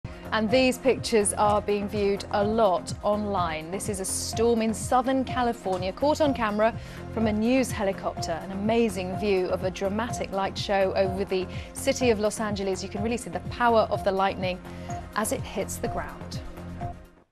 Weather Events 2019 - Dramatic lightning show over Los Angeles (USA) - BBC News - March 2019
A helicopter crew took these images of a dramatic lightning show over the city of Los Angeles in the USA.